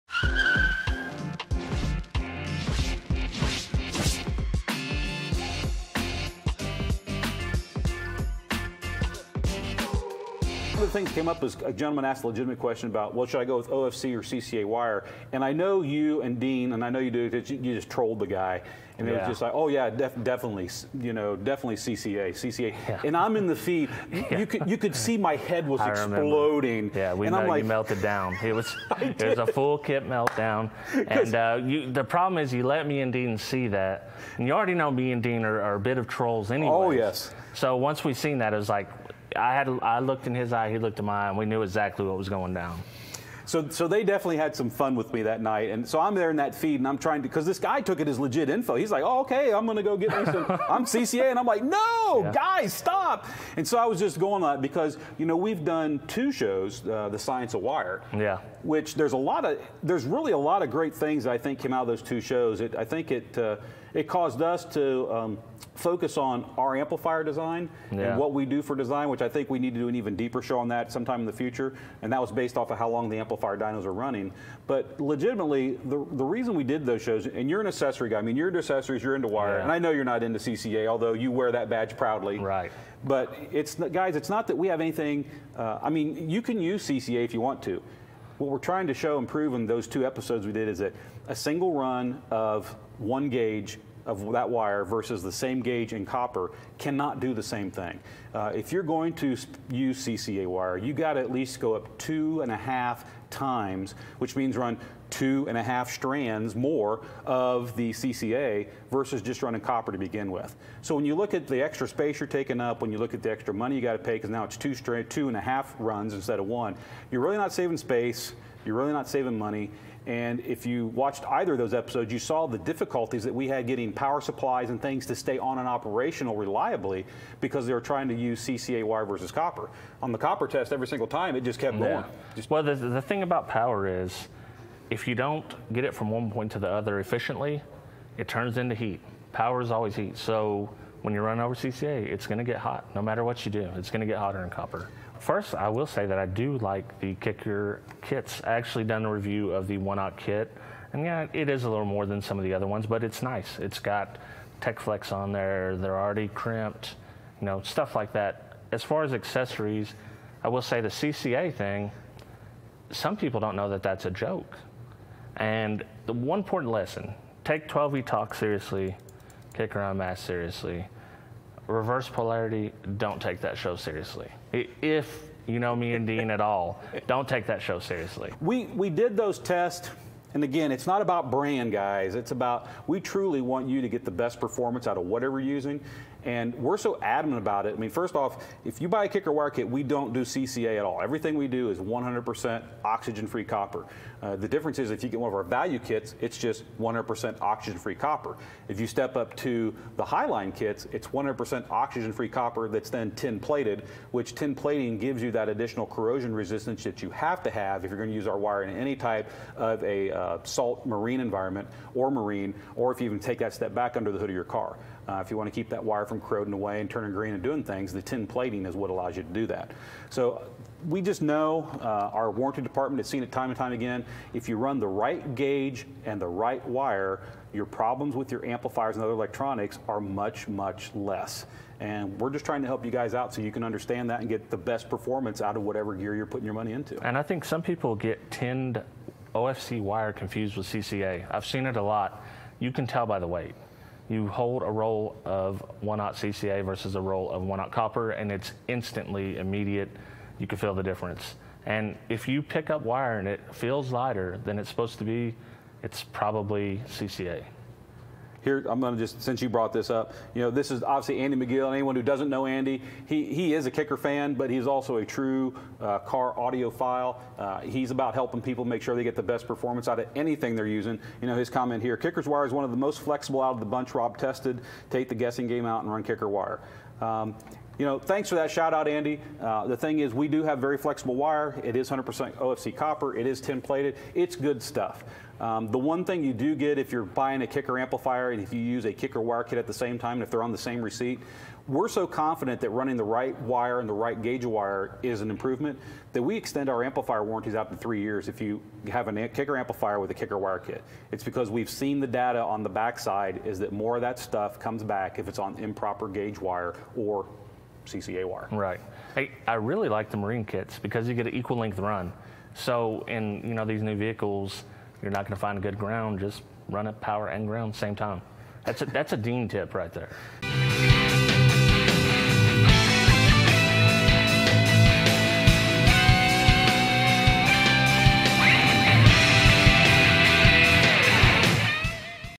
OFC vs. CCA- Which Wire is Best? Kicker UnMasked - Shorts
The efficiency of oxygen-free copper wire or the affordability of copper-clad aluminum...which has more advantages?  Kip and @HifiVega talk about the pros and cons of both!